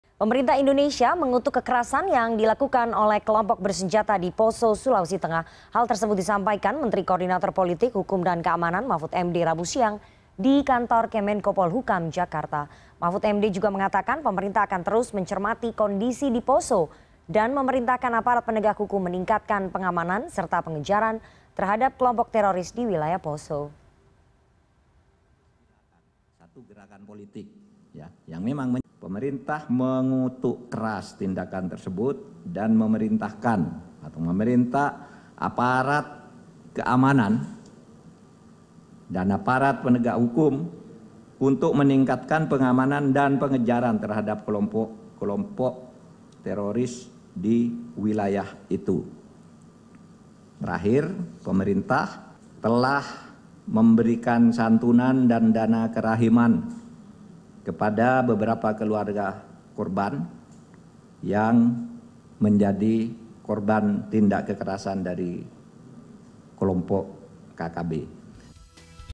Pemerintah Kutuk Kekerasan Oleh Kelompok Bersenjata di Poso
Pemerintah Indonesia, mengutuk kekerasan yang dilakukan oleh kelompok bersenjata, di Poso, Sulawesi Tengah.
Hal tersebut disampaikan Menteri Koordinator Politik, Hukum, dan Keamanan, Mahfud MD, rabu siang, di kantor Kemenko Polhukam, Jakarta.